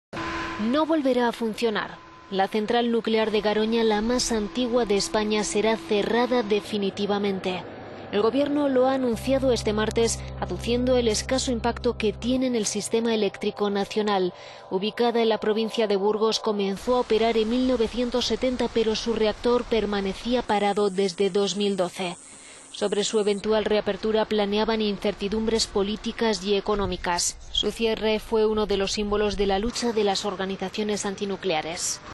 La central de Garoña no volverá a funcionar
La central nuclear de Garoña, la más antigua de España, será cerrada definitivamente. El Gobierno lo ha anunciado este martes aduciendo el escaso impacto que tiene en el sistema eléctrico nacional. Ubicada en la provincia de Burgos, comenzó a operar en 1970, pero su reactor permanecía parado desde 2012. Sobre su eventual reapertura planeaban incertidumbres políticas y económicas, ya que se necesitaban importantes inversiones para que siguiera activa. Su desmantelamiento, que se prolongará entre …